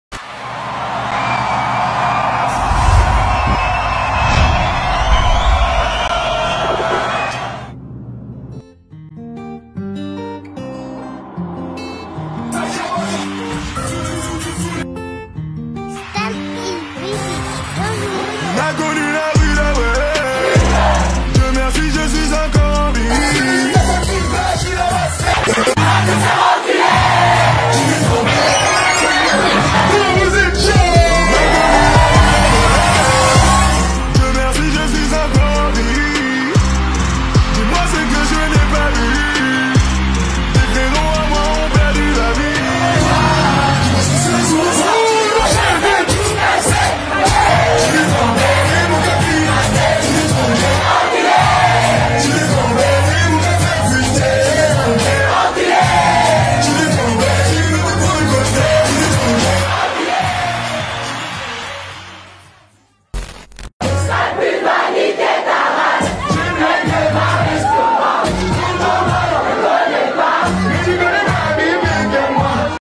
Beach de Tours Edition n°8 by Dj Djoka - 2018
Une nouvelle édition forte en émotion...

Showcase: Vegedream & Politik Naï